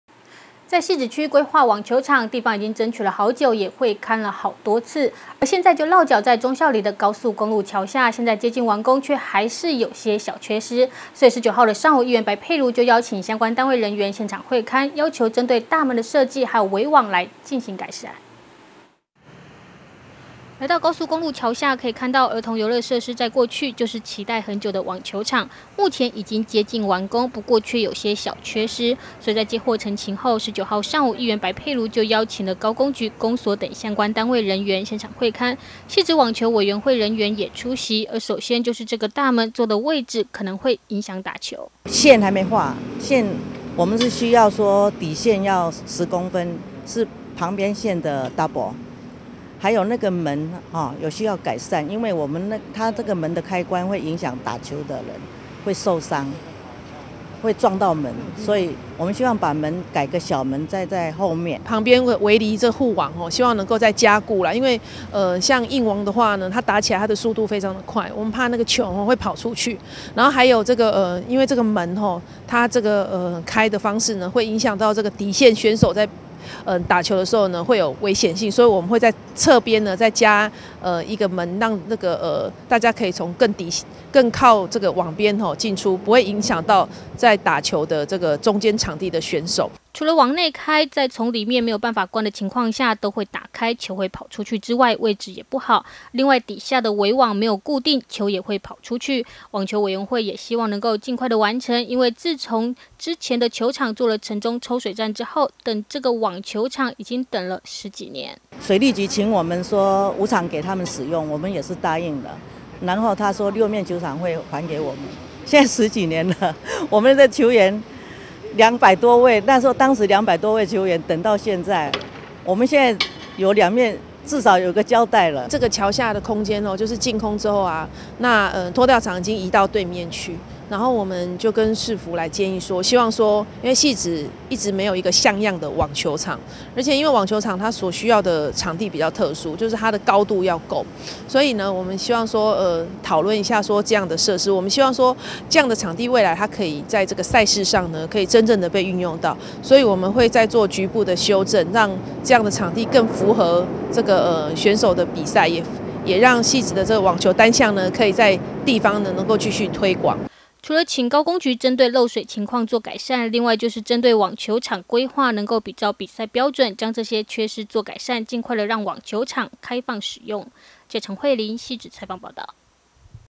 1060119觀天下新聞HD04 汐止區等十幾年 橋下網球場改缺失將開放
在汐止區規劃網球場，地方已經爭取了好久，也會勘了好多次，現在就落腳在忠孝里的高速公路橋下，接近完工，卻還有些小缺失，所以，19日上午，議員白珮茹就邀請了相關單位人員現場會勘，要求針對大門的設計及圍網，進行改善。